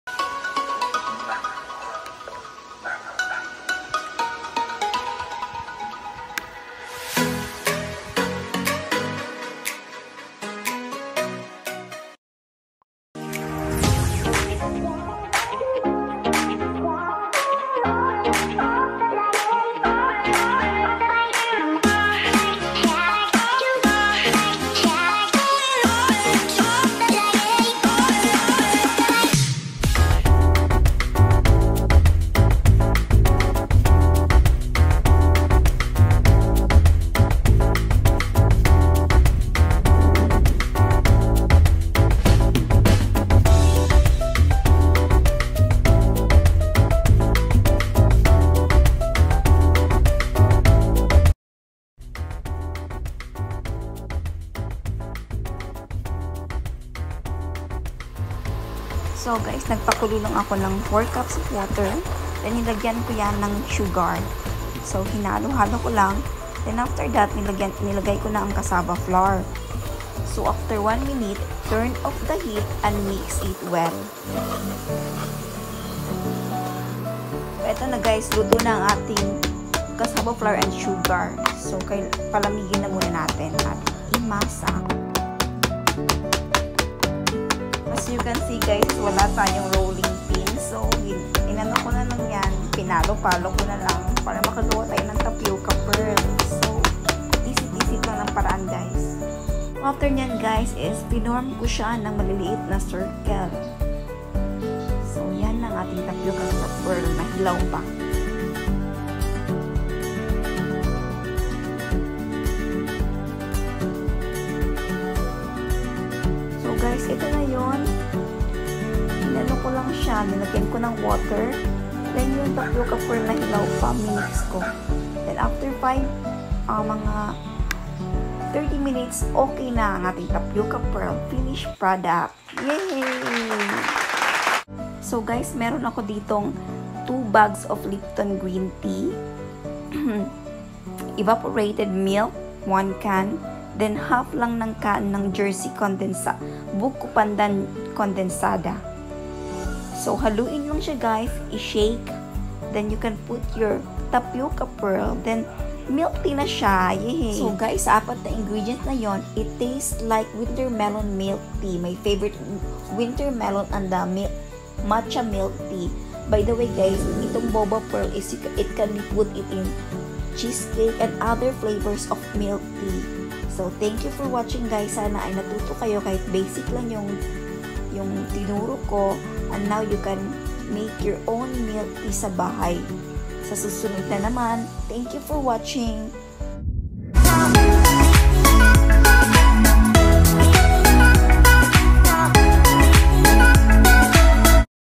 Homemade Tapioca Pearl|Philippines
Hello Guys! Mga kakikay! It’s quarantine day and I was craving for Wintermelon milktea. So I decided to make my own Milktea. And it was done perfectly! It’s more tasty because you can control all the ingredient. Mine was not so sweet.☺️ I really love it. 🥰😍